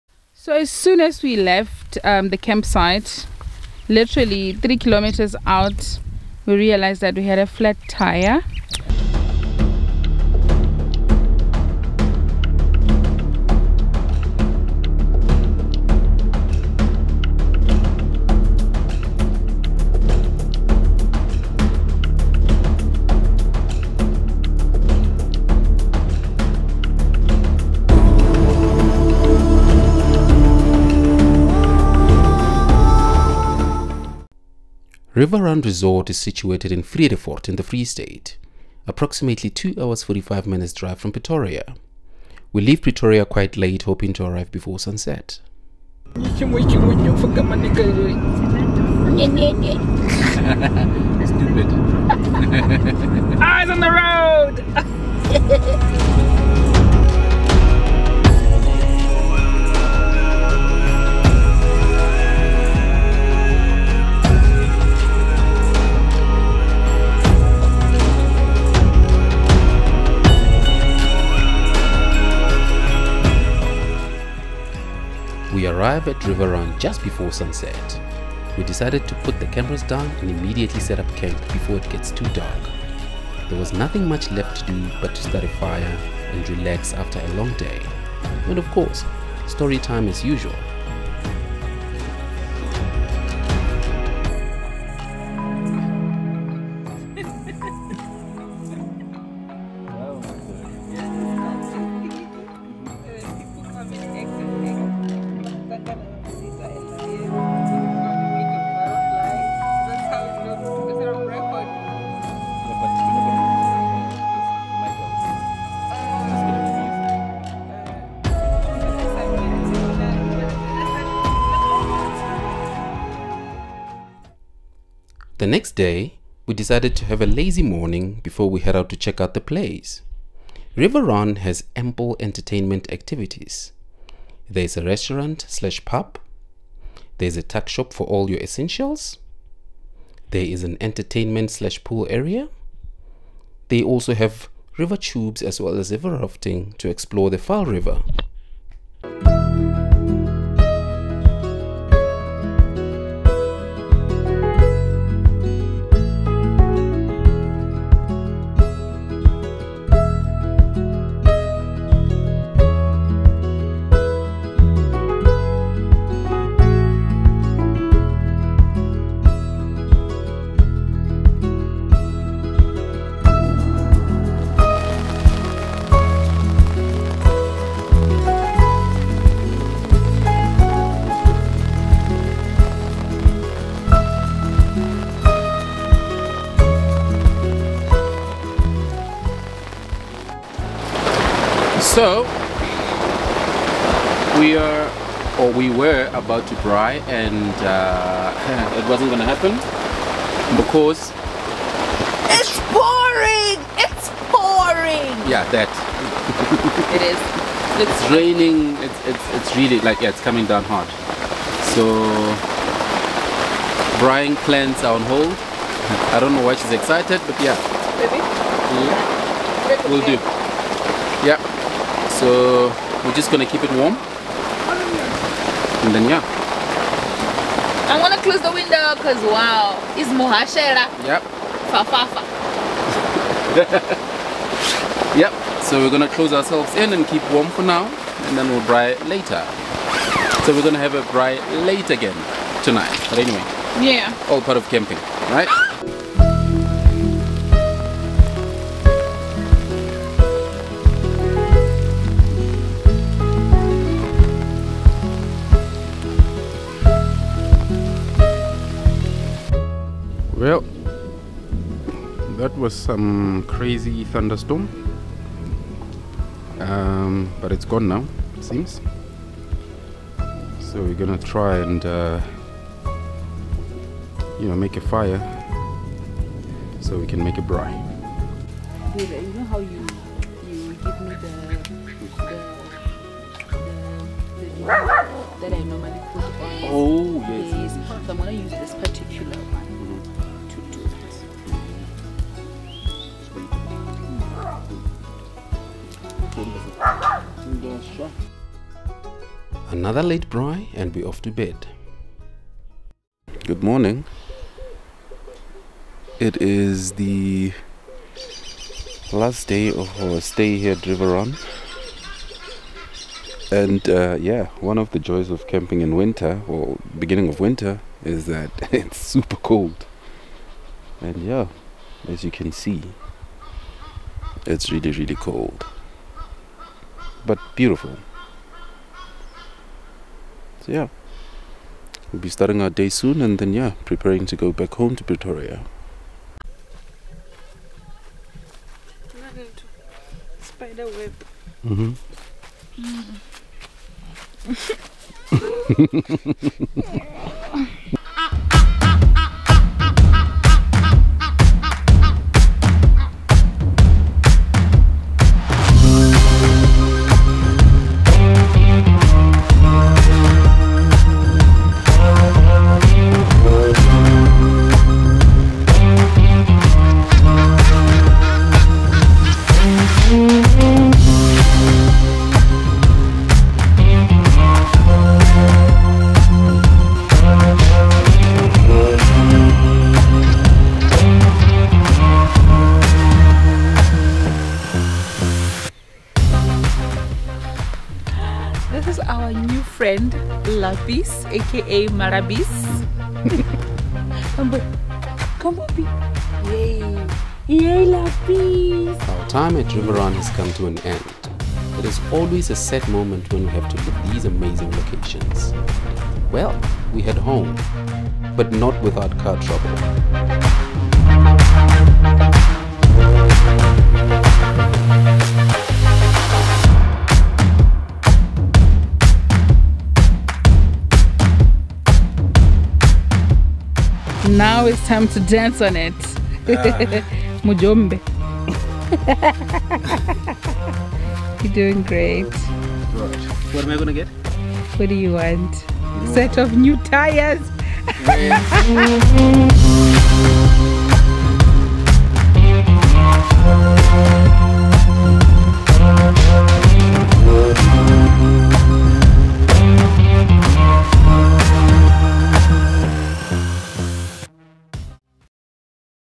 Camping on the banks of the Vaal river 📍 Riverrun
We go camping at Riverrun Resort in the Free State in the beginning of winter. Very cold nights, but very good location.